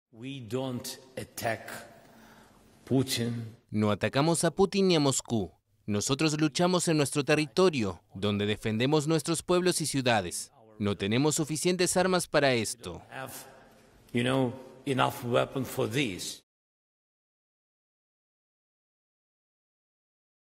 Zelenski niega que hayan intentado atacar a Vladimir Putin
El presidente ucraniano, Volodimir Zelenski, negó el viernes cualquier implicación de su país en una tentativa de asesinato de su homólogo ruso, Vladimir Putin, que según el Kemlin debía realizarse con drones que fueron derribados en Moscú.